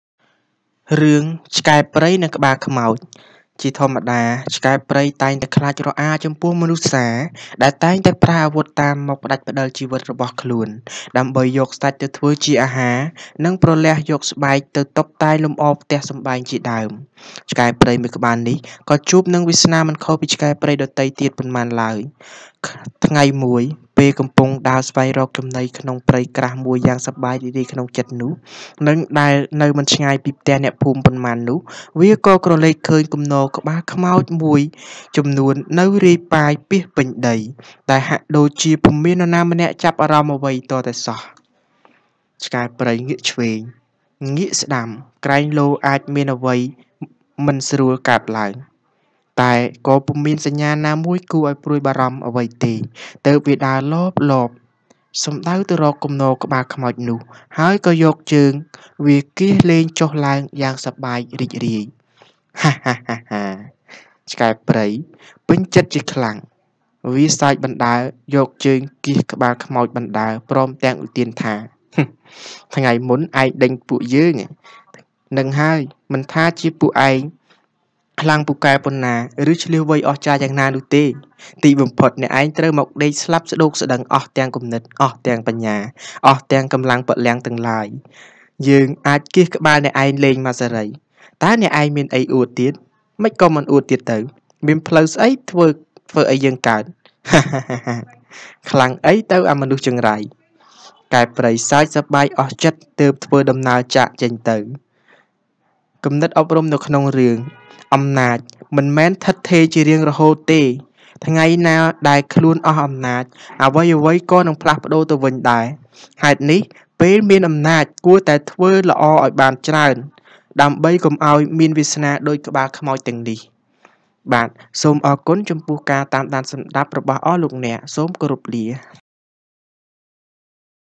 រឿង ឆ្កែព្រៃ និងក្បាលខ្មោច ,រយពាន់និទាន, រឿងនិទានដែលបង្កប់ដោយអត្ថន័យអប់រំសង្គម
"​ អំណាចមិនមែនស្ថិតស្ថេរជារៀងរហូតទេ ថ្ងៃណាដែលខ្លួនអស់អំណាច អ្វីៗក៏នឹងផ្លាស់ប្ដូរទៅវិញដែរ។ ហេតុនេះ ពេលមានអំណាច គួរតែខំធ្វើល្អឲ្យបានច្រើន ដើម្បីកុំឲ្យមានវាសនាដូចក្បាលខ្មោចទាំងនោះ "​